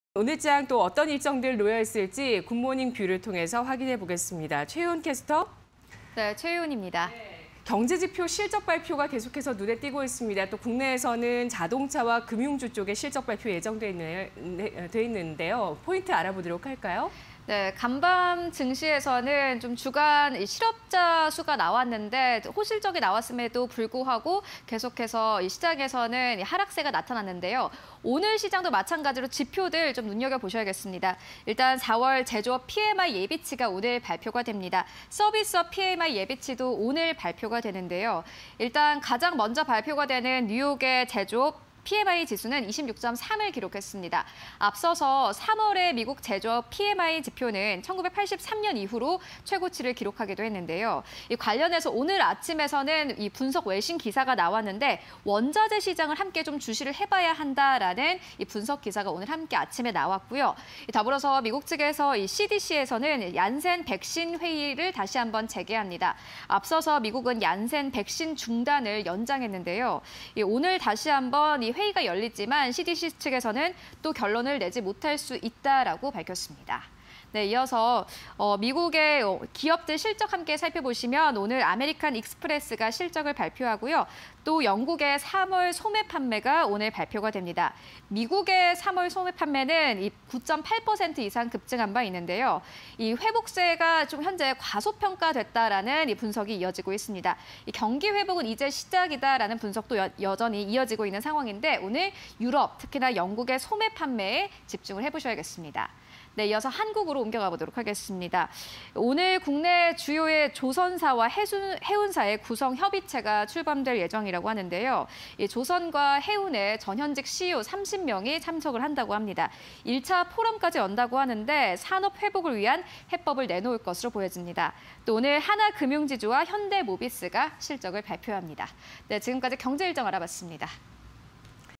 4월 뉴욕 연은 제조업PMI 지수 26.3_오늘장 글로벌 일정 (20210423)
미국 : 3월 신규주택매매
미국 : 4월 제조업 PMI 예비치
미국 : 4월 서비스업 PMI 예비치
미국 : CDC, 얀센백신 회의 재개

미국 : 4월 합성 PMI 예비치
미국 : 아메리칸 익스프레스 실적 발표
유럽 : 4월 합성 PMI 예비치
영국 : 3월 소매판매

한국 : 양자보안 스마트폰 갤럭시A퀀텀2 출시 예정
한국 : 국내 주요 조선사‧해운사 구성 협의체 출범
한국 : 하나금융지주 실적 발표
한국 : 현대모비스 실적 발표